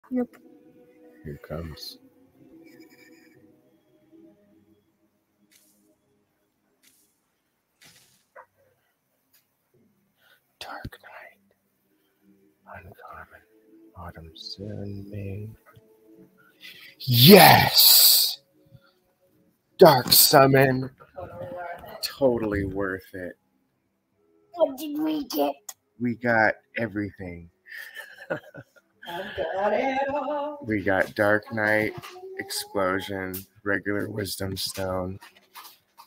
Burning a 1 of a kind Priest
Starbuck's gen1 burns a common one of a kind double explosion/double toxic priest with a rare DarkKnight using a regular insight stone(WIS)